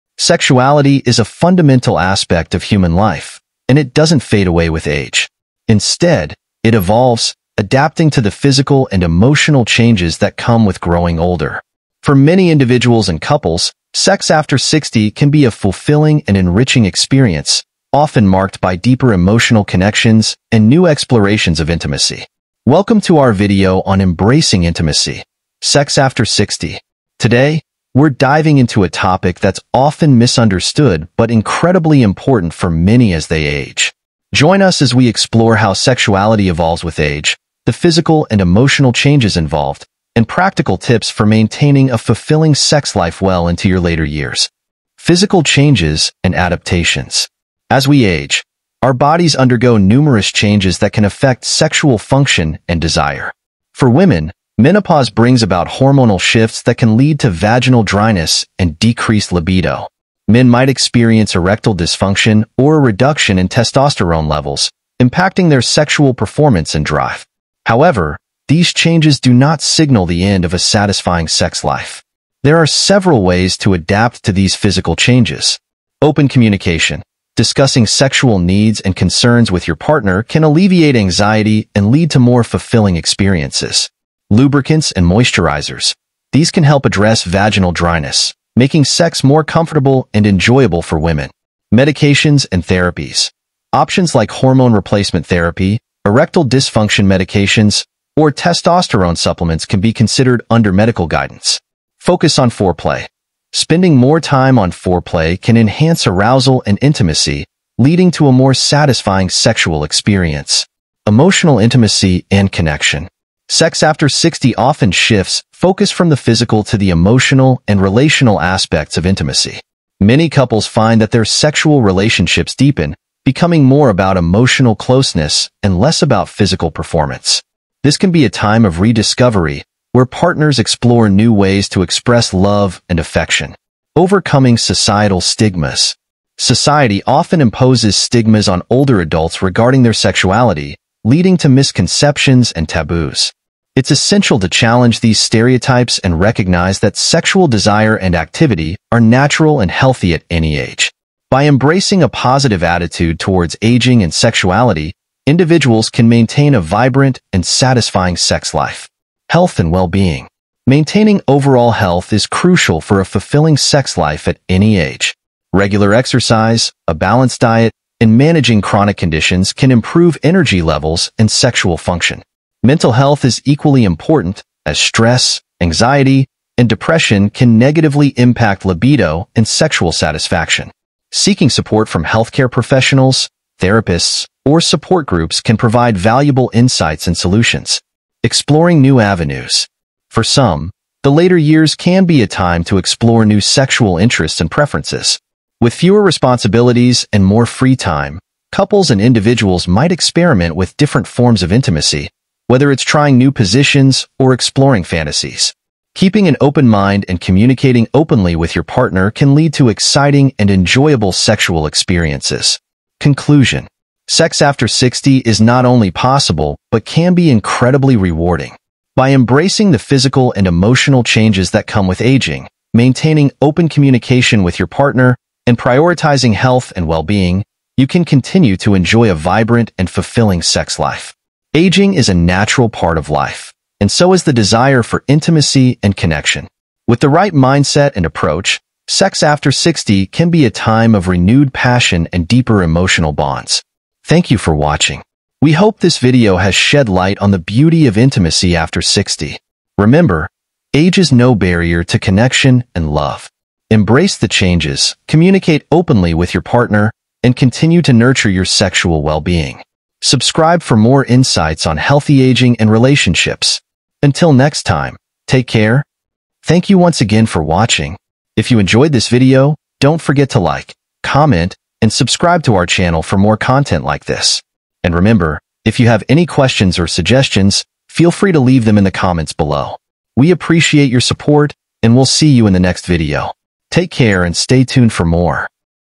What is sex like after 60?
Sex after 60 is a phase marked by a unique blend of physical changes and emotional depth. As bodies age, hormonal shifts and physical adaptations such as vaginal dryness or erectile changes may occur, but these challenges can be navigated with openness and communication. The focus often shifts from performance to emotional connection, fostering deeper intimacy between partners. Overcoming societal stigmas about aging and sexuality is crucial, allowing individuals to embrace their desires and explore new forms of intimacy. By prioritizing overall health and well-being through exercise, diet, and managing stress, older adults can maintain a vibrant sex life. This stage of life offers opportunities for exploring new interests and fantasies, ensuring that sexual fulfillment remains an enriching aspect of later years.